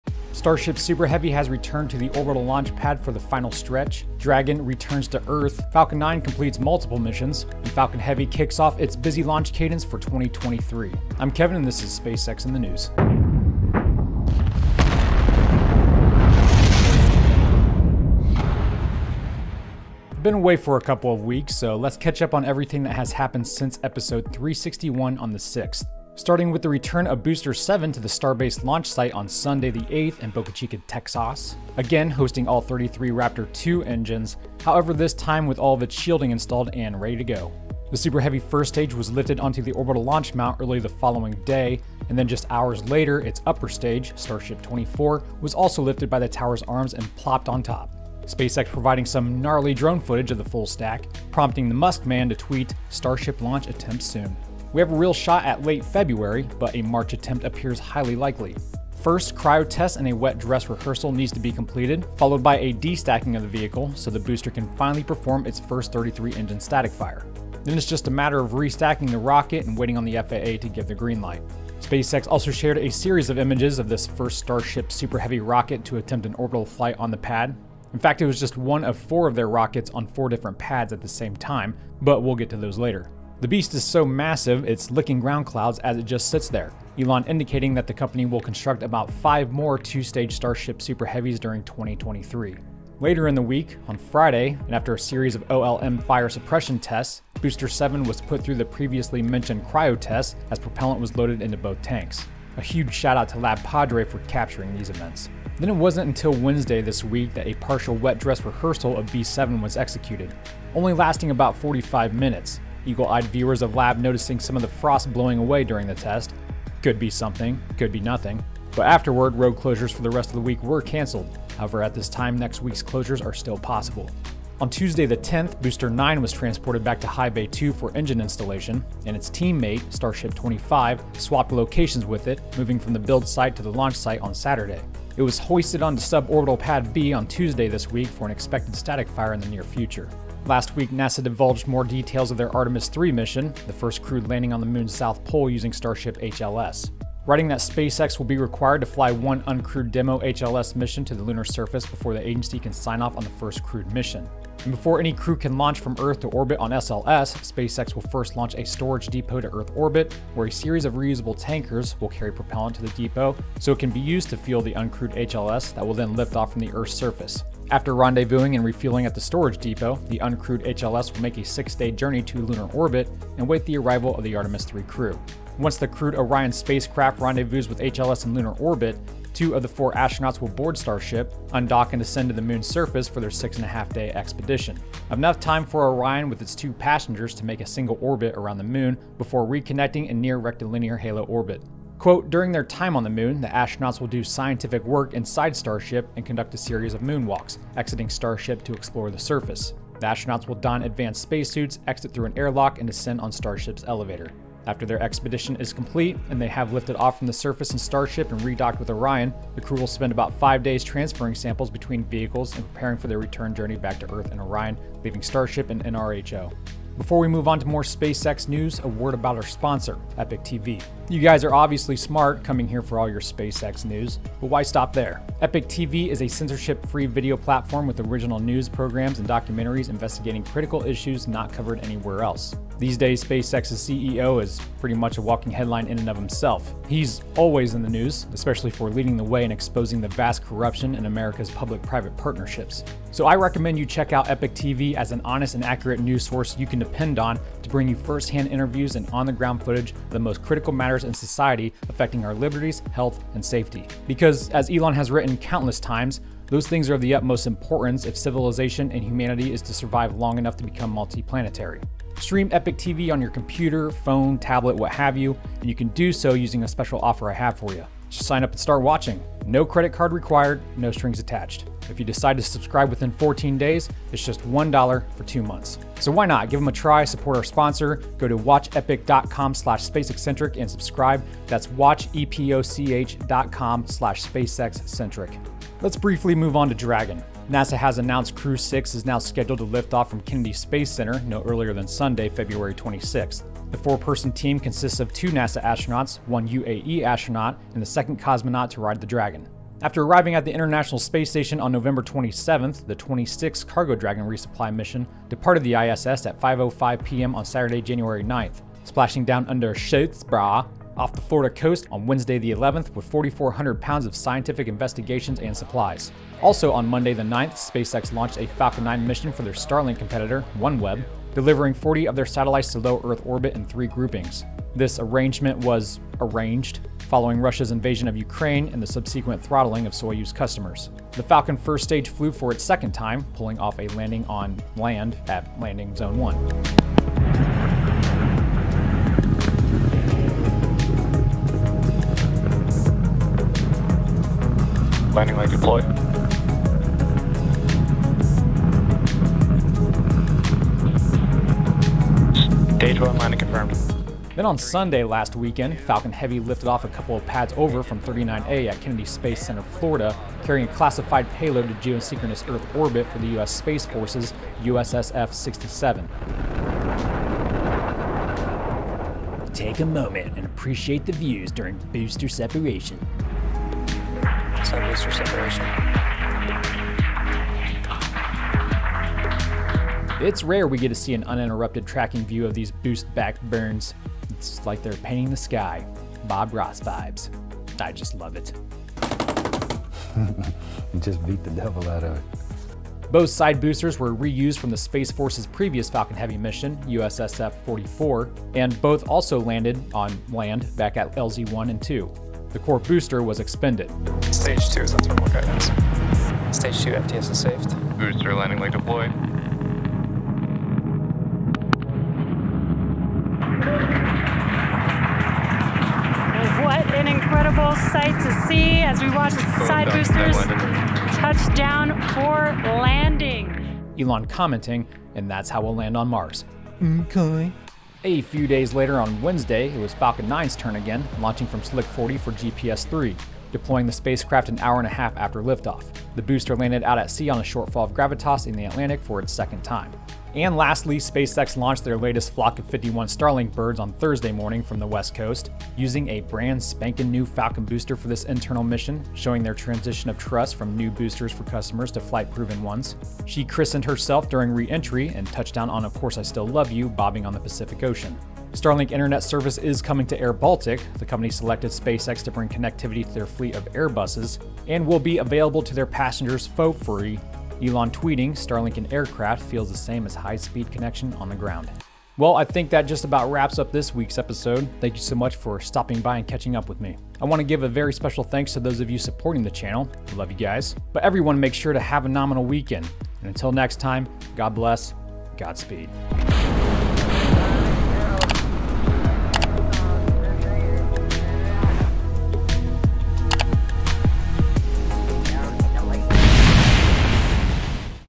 SpaceX Starship Full Stack Enters Final Stretch Before First Orbital Flight, HLS Details Revealed
Watch unbiased, truthful news in Epoch TV on any device.

Starship Super Heavy has returned to the orbital launch pad as it closes in on the first orbital launch attempt. Dragon makes a splashdown. Falcon 9 completes multiple missions. And Falcon Heavy kicks off its busy launch cadence for 2023. Ep 362

THUMBNAIL: SpaceX

TIMESTAMPS:
Intro: 0:00
Starship: 0:20
Dragon: 4:45
Falcon Missions: 5:15
Starlink: 7:20